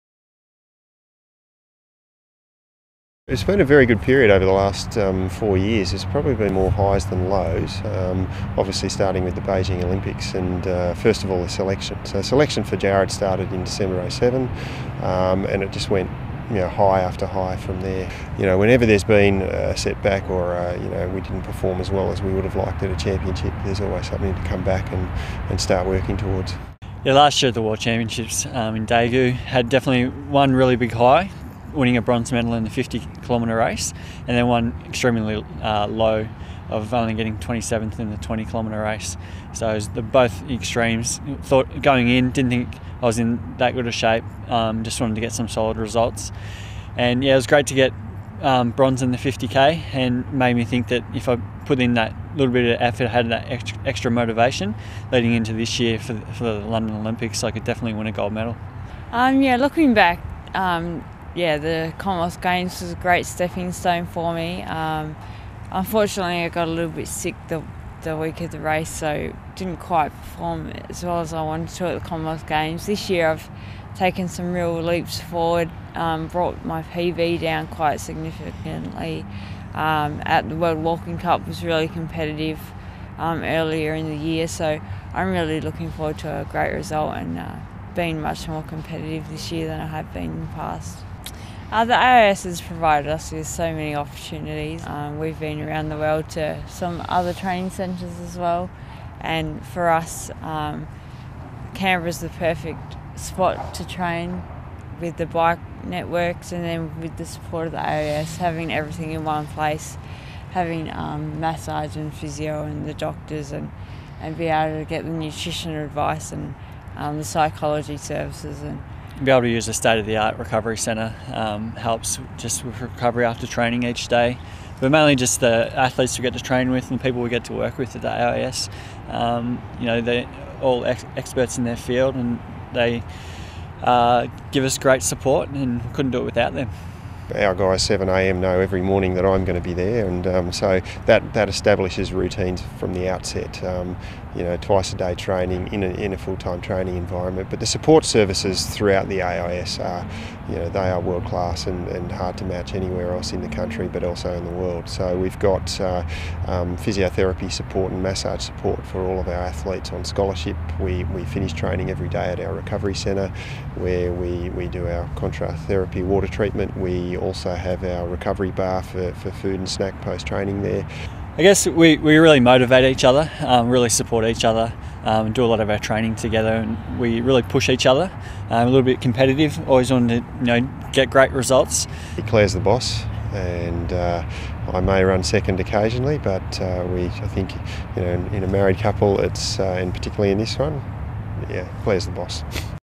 Interview with Jared and Claire Tallent
Interview with Jared and Claire Tallent before the London Olympics